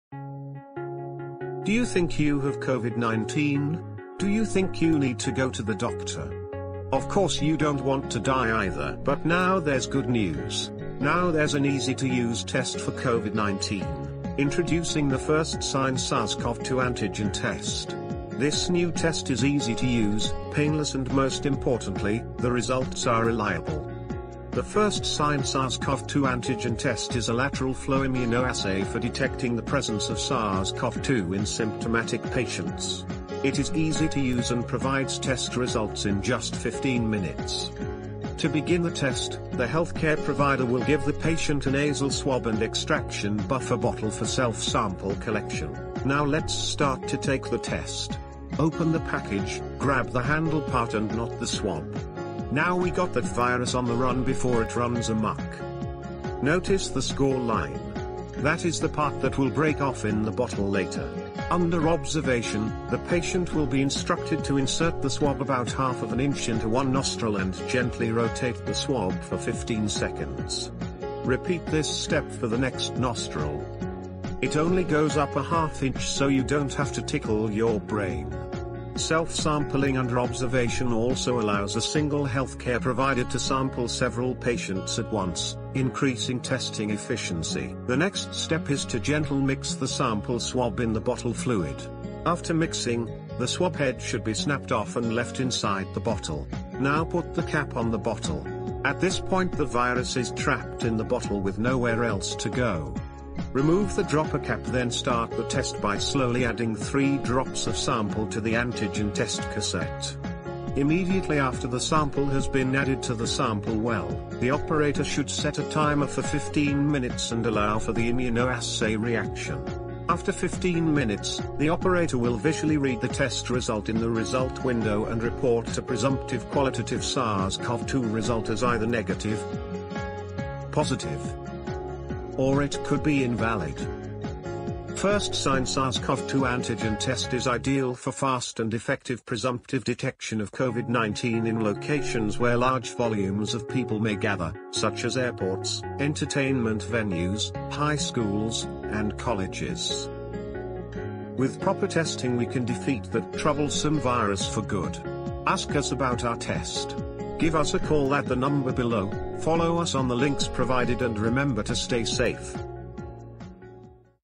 How to Take a COVID 19 Antigen Test CARTOON
The First Sign® SARS-CoV-2 Antigen Test was designed with the Point-of-Care in mind. A simple to use, easy to understand testing process is key to the widespread adoption and correct application of SARS-CoV-2 rapid antigen tests. The First Sign® was developed with a high sensitivity bias, guaranteeing a reliable Limit of Detection (LoD), increasing total sensitivity, and guarding against false-negative results.